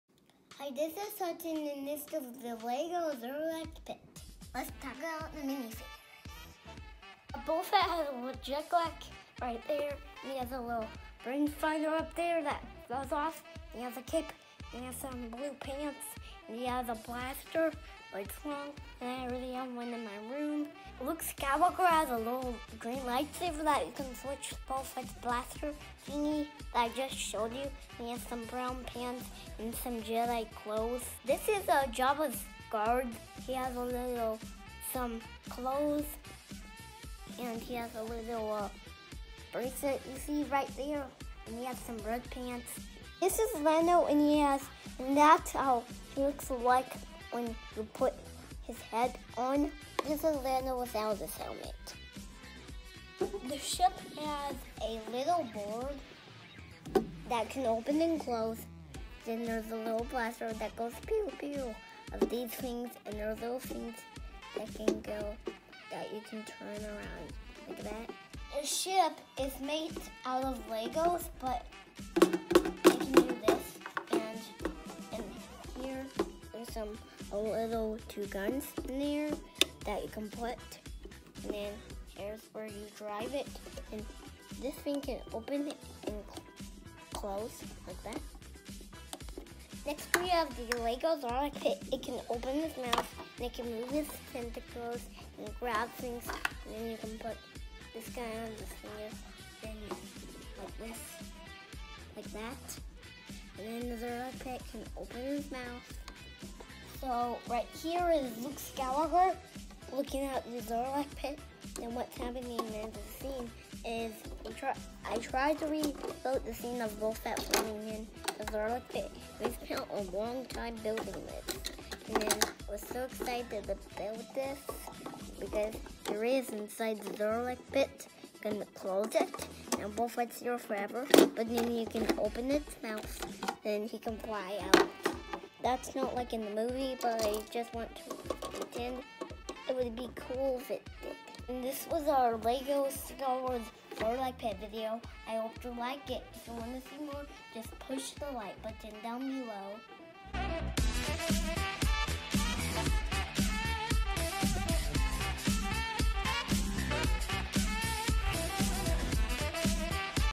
Lego Star Wars Desert Skiff - 9496 Sarlacc Pitt Review
Hudson reviews the 2012 Lego Star Wars Desert Skiff (9496) featuring the Sarlacc Pit!